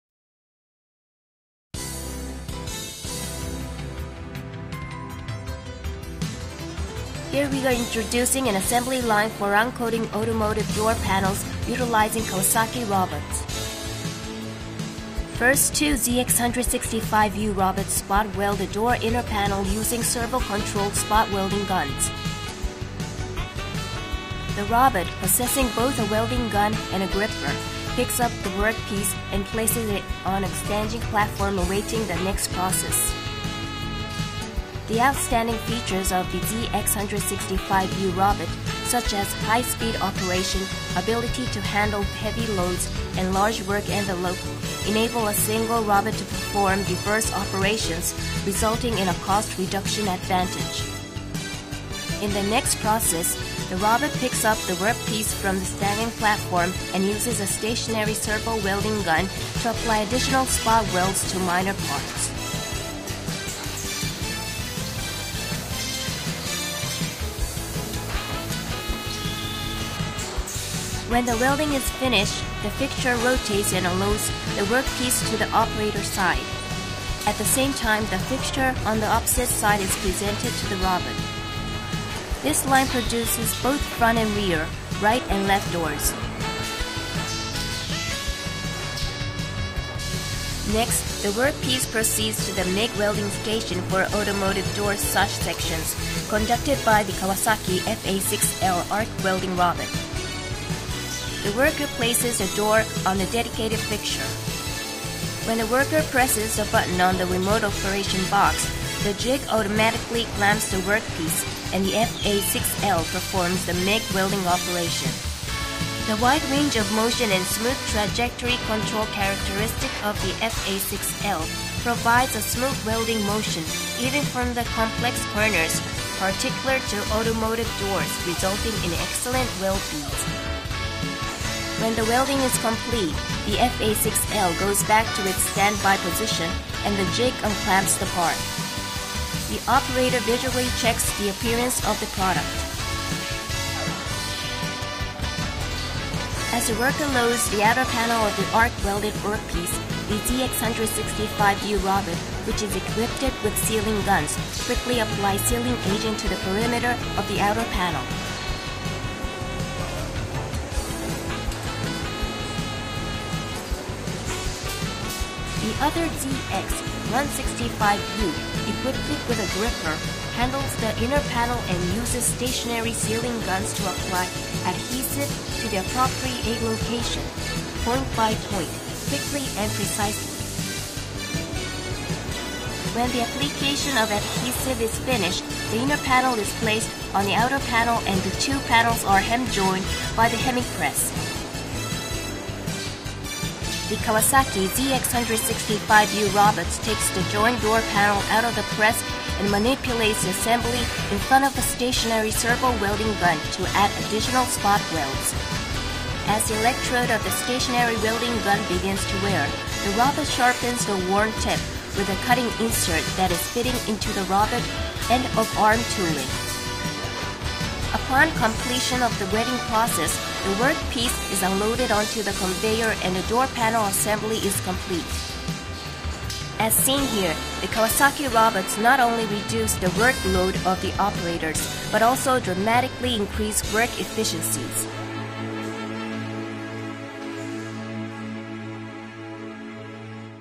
Assembling an Automotive Door Panel - Kawasaki Robotics
Door panel assembly line with the Kawasaki FA06L and ZX165U robots.